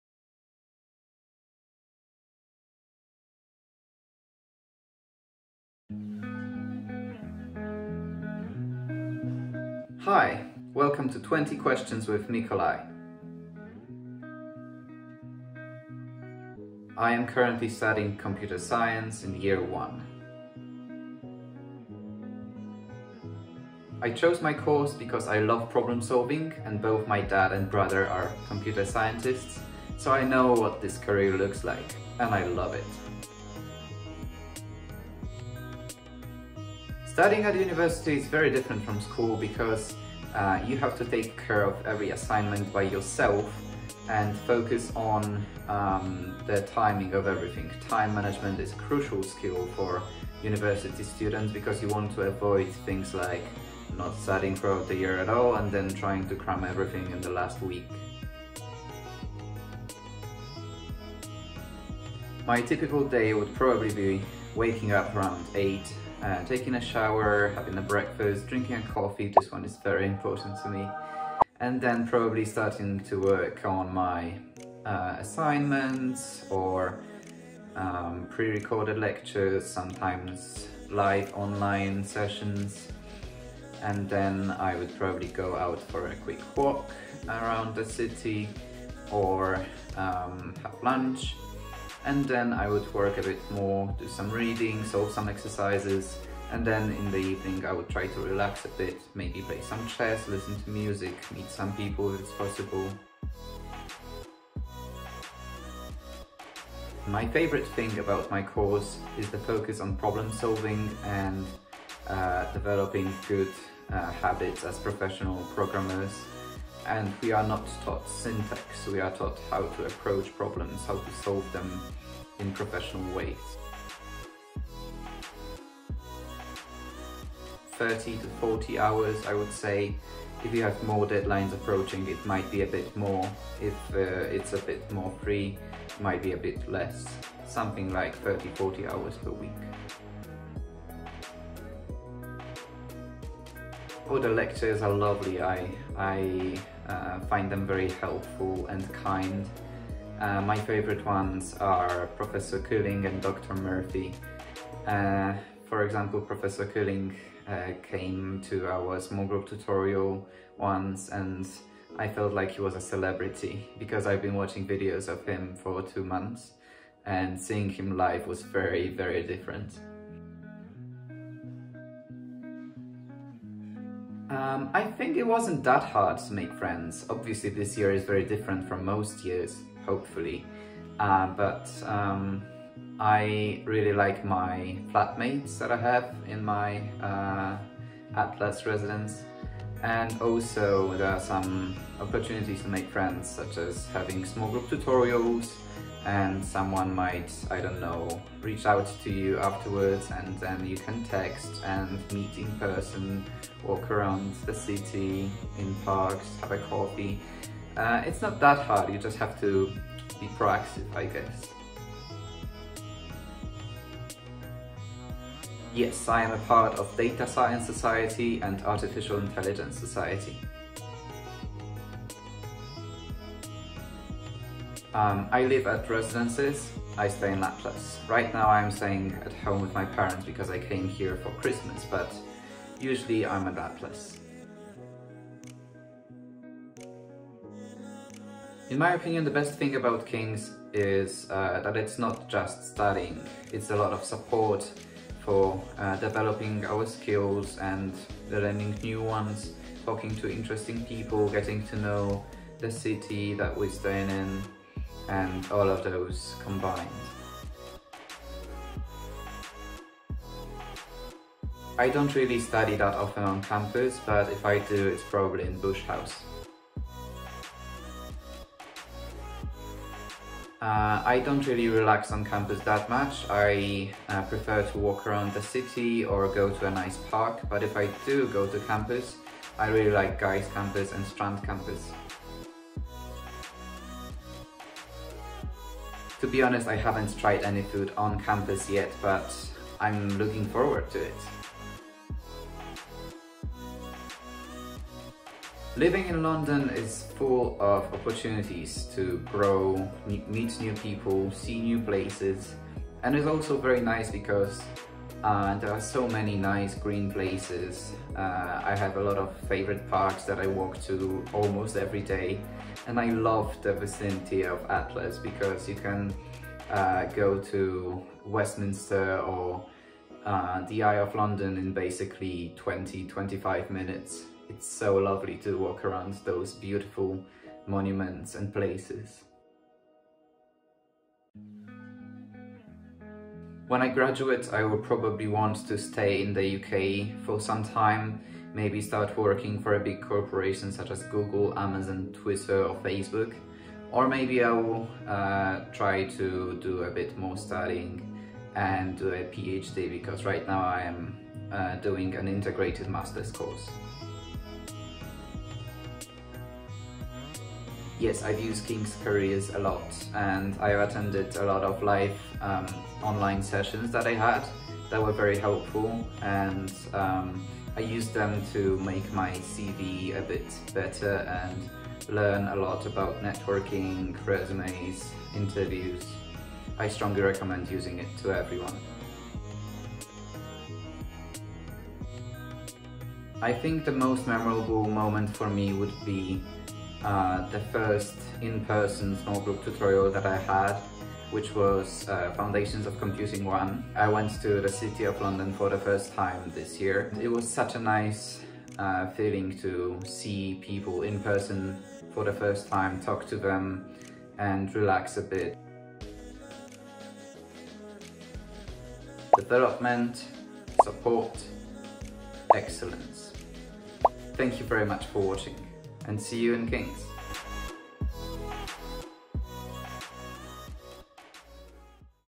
Computer Science BSc at King's | 20 questions with Mikolaj
Find out about what it's like to study Computer Science at King's with first-year student Mikolaj.

Timestamps:

00:14 – What’s your course?

00:20 – Why did you choose to study your course?

00:34 – How I studying at university different from school?

00:59 – What’s a typical day like?

01:44 – What do you like most about your course?

02:04 – How many hours do you study a week?

02:21 – What are the academics like?

02:50 – Was it easy to make friends?

03:32 – Are you part of any societies?

03:42 – Do you live at Home or in Halls?

03:55 – What’s the best thing about King’s?

04:19 – Where do you go to study on campus?

04:27 – Where do you go to relax on campus?

04:42 – Favourite place to eat on campus?

04:52 – What’s it like living in London?

05:29 – What do you want to do when you graduate?

05:55- Have you King’s Career and Employability Centre?

06:20 – What’s your most memorable moment at King’s so far?

06:46 – Describe King’s in 3 words?